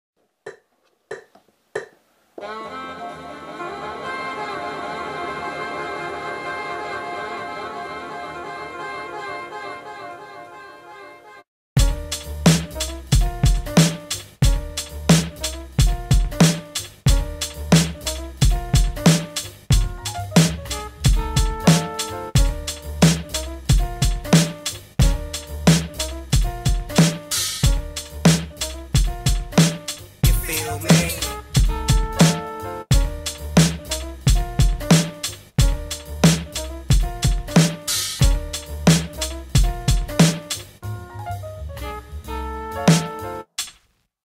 AKAI MPD 226 Test - Jazzy Instrumental
Finally decided to invest in one of these, definitely having fun with it so far! Thought I'd share the first beat I did on it, a quick jazzy instrumental sampling Miles Davis and a 2pac vocal chop, running the MPD in FL studio.

Looking forward to getting more familiar with this, including it in my productions and learning to play my music live!